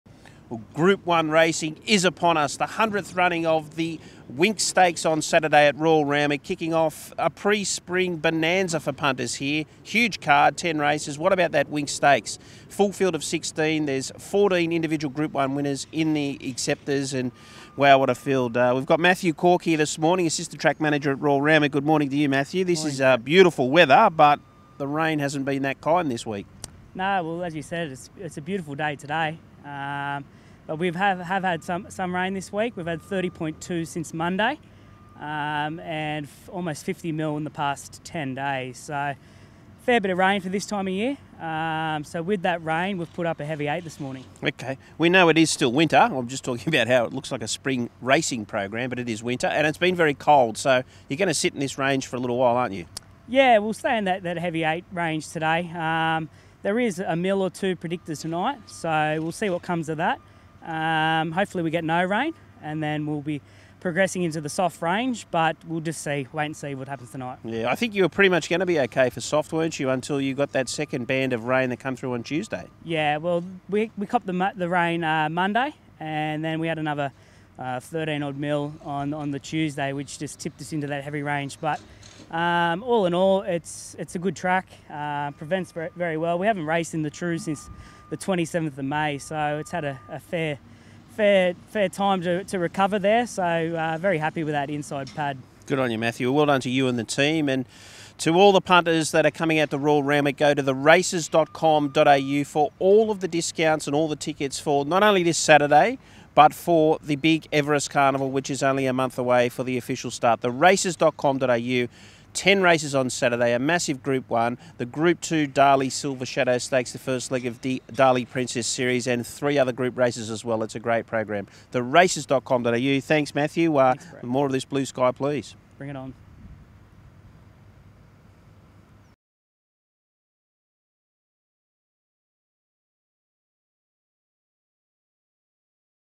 Royal Randwick Track Report | Winx Stakes Day 2023
The Group 1 $1 million Winx Stakes run in honour of Australia’s immortal racehorse headlines a huge day of Group racing at Royal Randwick this Saturday before the official start of Spring’s Sydney Everest Carnival.

A field of 16 and three emergencies have accepted for the 100th running of the $1 million Winx Stakes, which this year increases in prizemoney as part of all Group races in NSW rising to new minimum levels across the season.

Saturday’s meeting features four other Group races including the first leg of the Darley Princess Series for three-year-old fillies the Group 2 $300,000 Darley Silver Shadow Stakes.